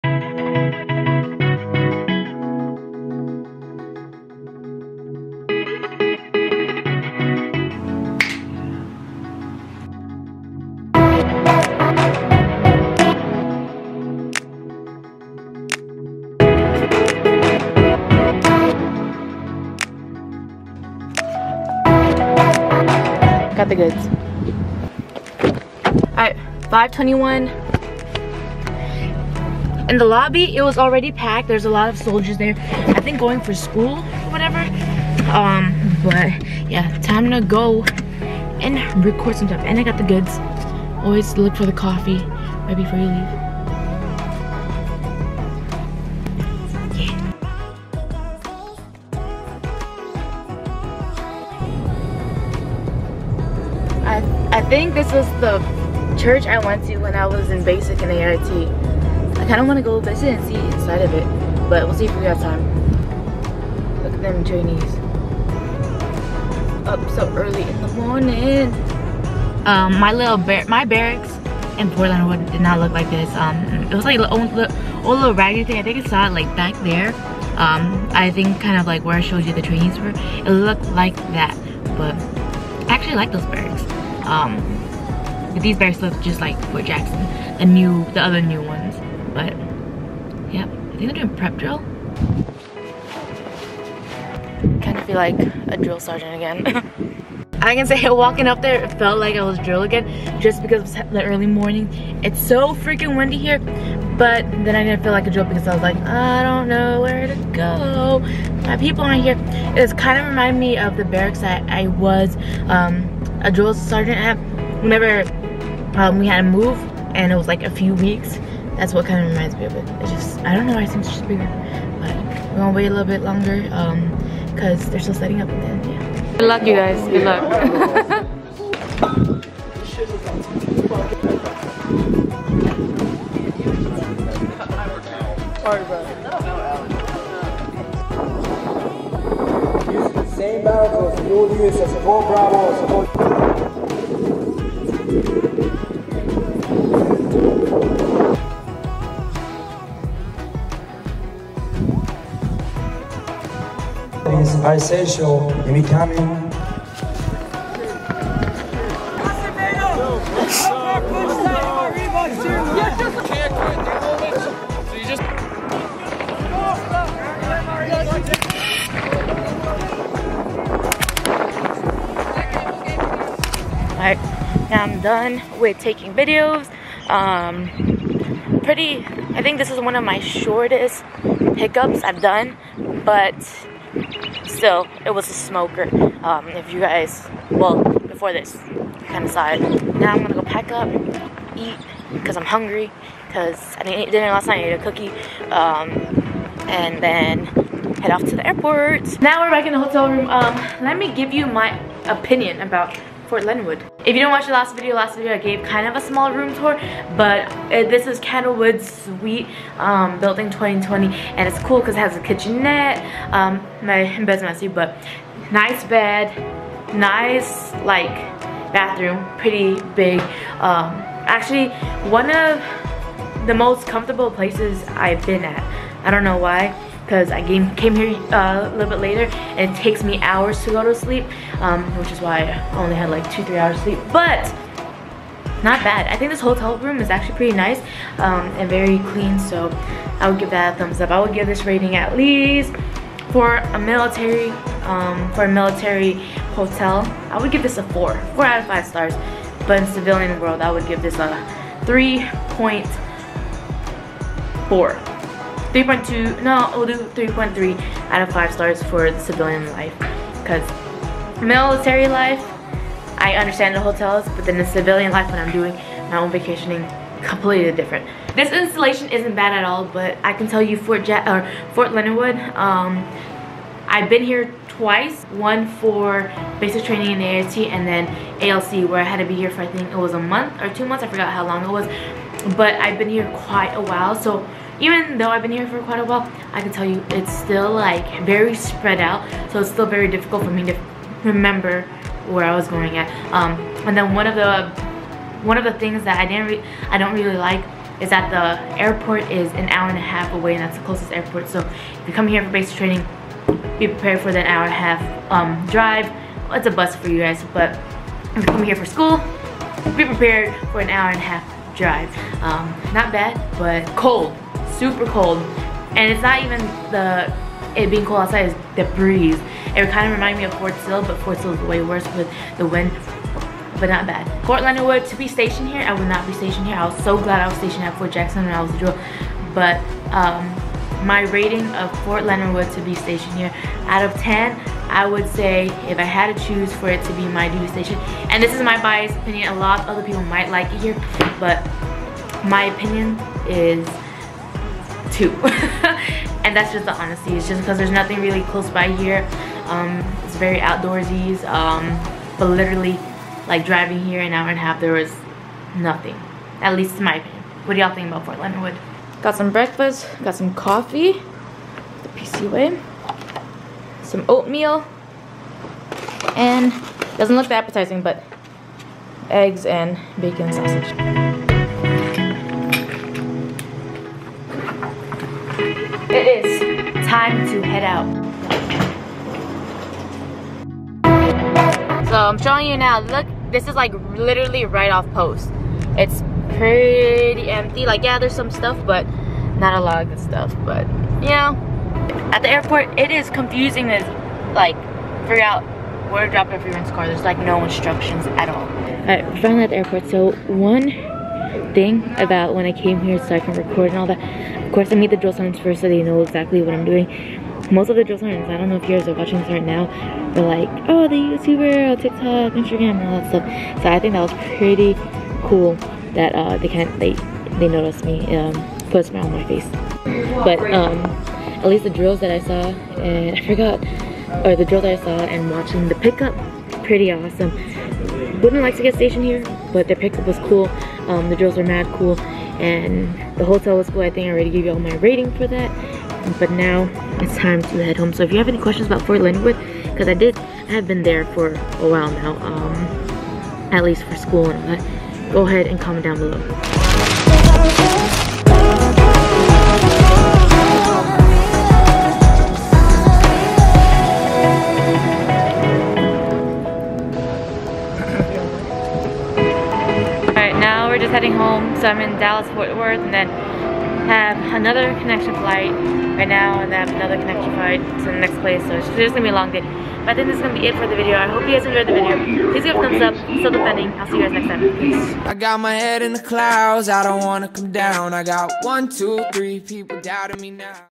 Day in the Life of Fort Leonard Wood Basic Training Pick Up | Filming
Another trip to another installation! I had the opportunity to visit Fort Leonard Wood, Mo for basic training pick up. This is home to the chemical corps, so I've been there before, but this was for Basic training. A different side of things. Have you been stationed here before? What do you think?

Products I use:
Everyday Vlogging Camera
Secondary Camera
Small Camera
The laptop I use